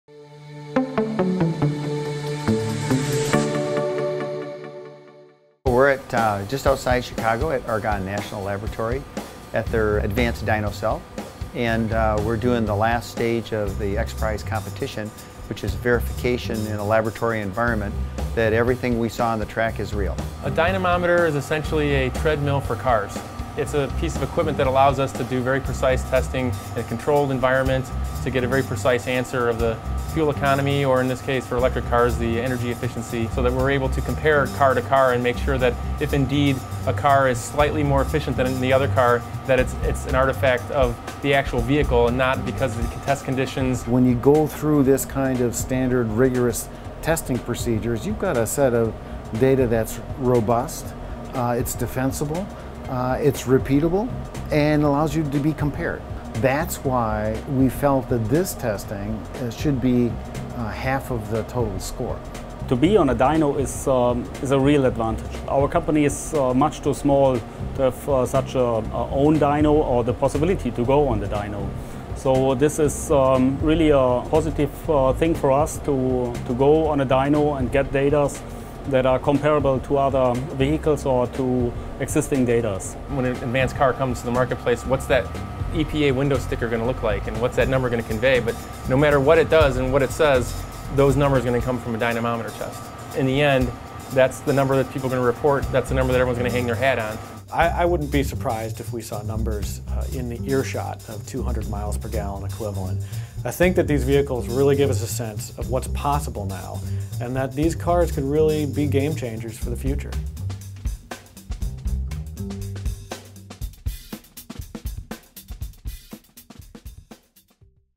Validation - Progressive Insurance Automotive X PRIZE
The teams have moved onto the Validation stage.  In this final technical event, the top finalists in both the Mainstream and Alternative classes will undergo dynamometer testing under controlled laboratory conditions at certified labs to verify technical performance results. The Validation stage accounts for 50% of the overall score for each team.  The results of this testing, combined with the speed, efficiency and emissions results from the earlier events at Michigan International Speedway, will determine the winners of the Progressive Insurance Automotive X PRIZE.

GET INVOLVED!